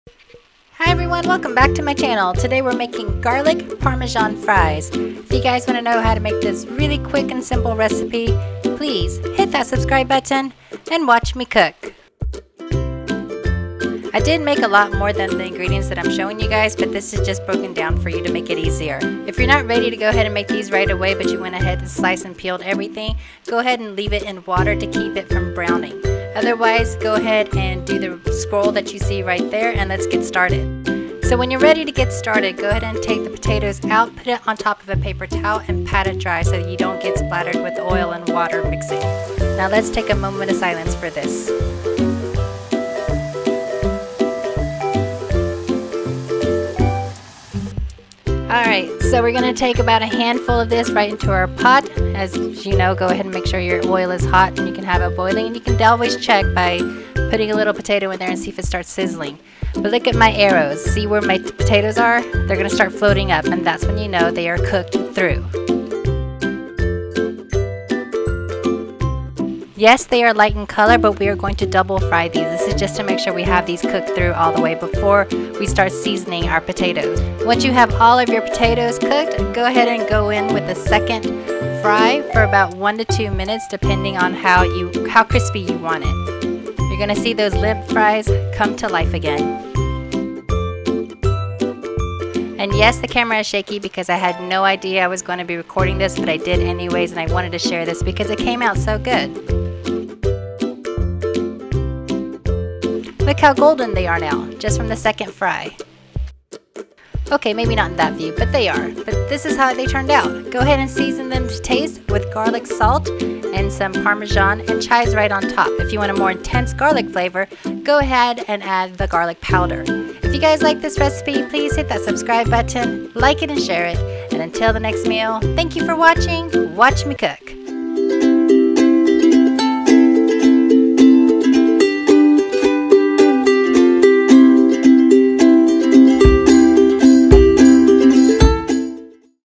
Garlic Parmesan Fries WatchMiCook Episode 118
A super simple and fast garlic parmesan fries video just for you!

Use whatever ingredients you have on and this is easily adjustable.
I used an Idaho potato but you can use a sweet potato as well too.

You know those parm packs you get from a pizza joint? Yes, that's what I used. Season to everything to taste and voila! An easy recipe to snack on!

Sorry about the shakiness. I explained that I had no planned on recording this video and everything was recorded without my stand. But its super fast video.

~WatchMiCook

or